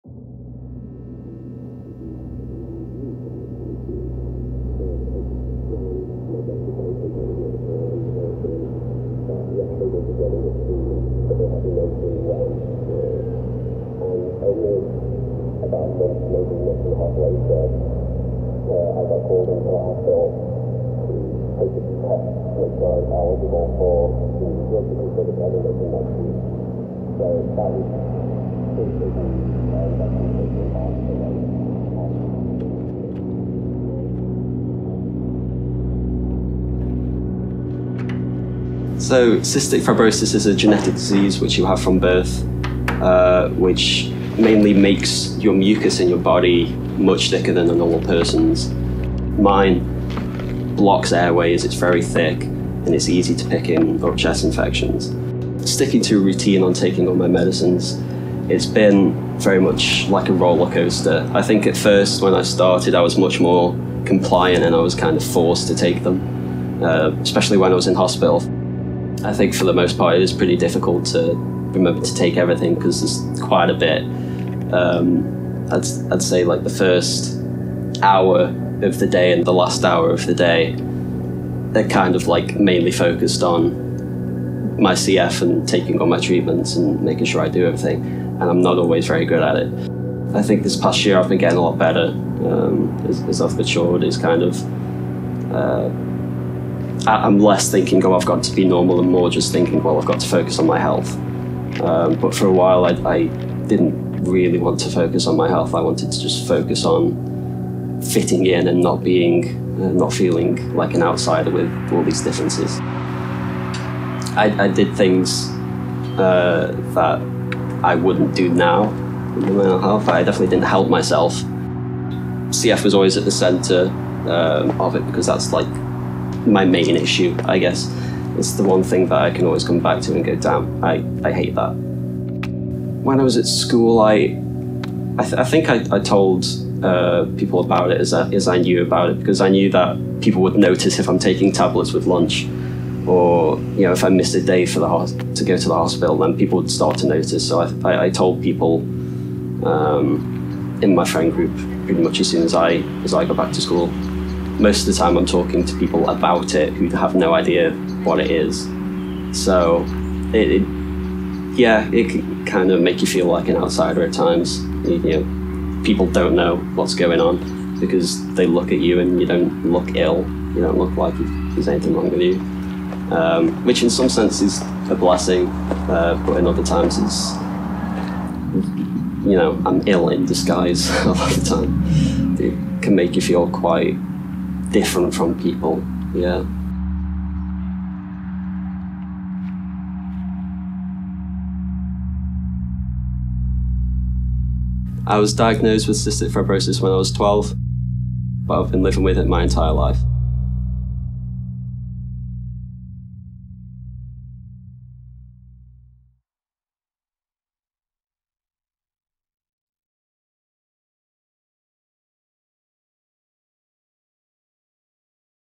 CFWilshaw
Short documentary film to bring awareness to Cystic Fibrosis

Made By Crew I: Jack Pedley, Ekate Trufanava and Phoebe Wilson
Soundtrack by: Cameron Tura